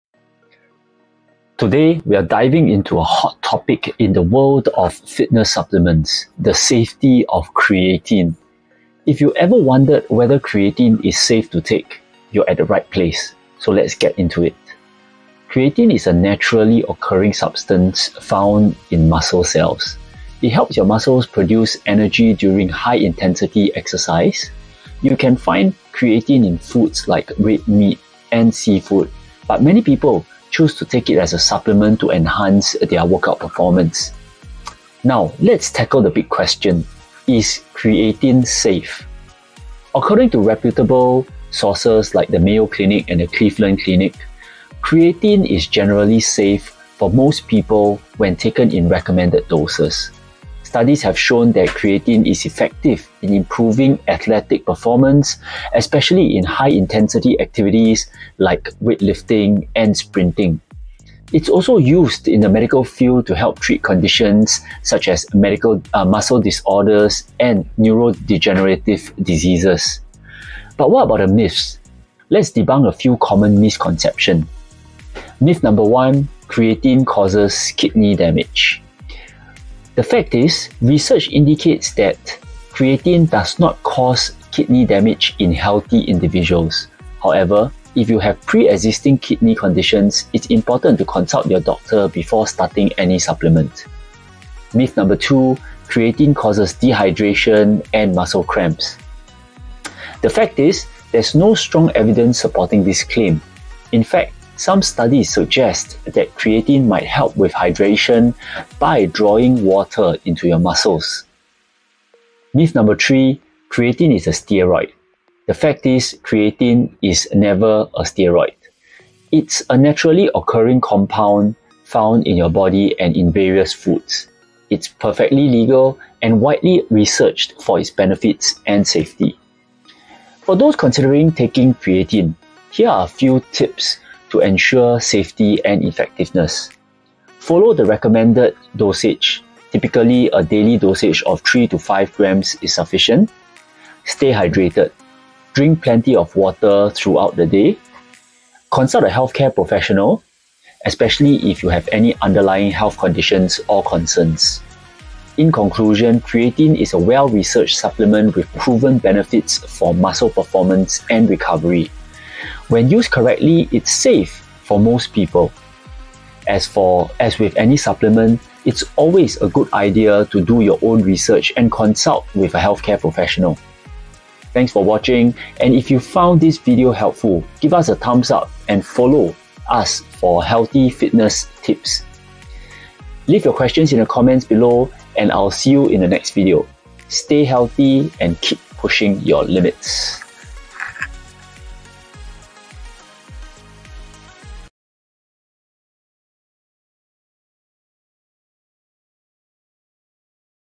Is Creatine Safe? Everything You Need to Know About Creatine Supplements
Discover the safety of creatine supplements in our latest video! We delve into the benefits, potential side effects, and scientific research behind creatine usage. Learn if creatine is safe for you, how it affects your body, and the best practices for its use.

Today, we’re diving into a hot topic in the world of fitness supplements – the safety of creatine. If you’ve ever wondered whether creatine is safe to take, you’re in the right place. Let’s get into it!
Creatine is a naturally occurring substance found in muscle cells. It helps your muscles produce energy during high-intensity exercise. You can find creatine in foods like red meat and seafood, but many people choose to take it as a supplement to enhance their workout performance.
Now, let’s tackle the big question: Is creatine safe? According to reputable sources like the Mayo Clinic and Cleveland Clinic, creatine is generally safe for most people when taken in recommended doses.
Studies have shown that creatine is effective in improving athletic performance, especially in high-intensity activities like weightlifting and sprinting. It’s also used in the medical field to help treat conditions such as muscle disorders and neurodegenerative diseases.
But what about the myths? Let’s debunk a few common misconceptions.
Myth #1: Creatine causes kidney damage.
Fact: Research indicates that creatine does not cause kidney damage in healthy individuals. However, if you have pre-existing kidney conditions, it's important to consult your doctor before starting any supplement.
Myth #2: Creatine causes dehydration and muscle cramps.
Fact: There’s no strong evidence supporting this claim. In fact, some studies suggest that creatine might help with hydration by drawing water into your muscles.
Myth #3: Creatine is a steroid.
Fact: Creatine is not a steroid. It’s a naturally occurring compound found in your body and in various foods. It’s perfectly legal and widely researched for its benefits and safety.
For those considering taking creatine, here are a few tips to ensure safety and effectiveness:
Follow the recommended dosage: Typically, a daily dose of 3-5 grams is sufficient.
Stay hydrated: Drink plenty of water throughout the day.
Consult a healthcare provider: Especially if you have any underlying health conditions or concerns.
In conclusion, creatine is a well-researched supplement with proven benefits for muscle performance and recovery. When used correctly, it’s safe for most people. As with any supplement, it’s always a good idea to do your own research and consult with a healthcare professional.

00:00 Intro
00:16 What is Creatine?
00:38 Is Creatine Safe?
01:15 Myth #1
01:35 Myth #2
01:53 Myth #3
02:15 Tips Of Taking Creatine